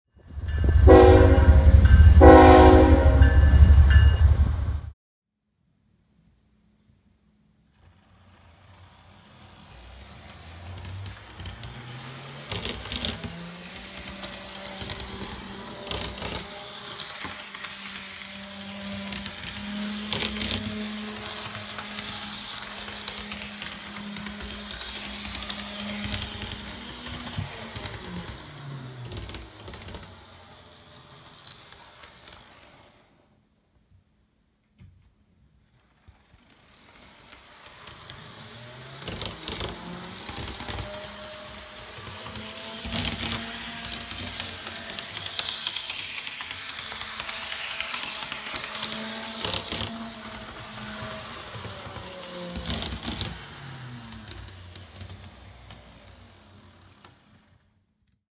N Scale Life Like Southern Railway 2909 EMD E7 Diesel Locomotive Demo
Demonstration video of product for sale on Apex Model Train Co. eBay store.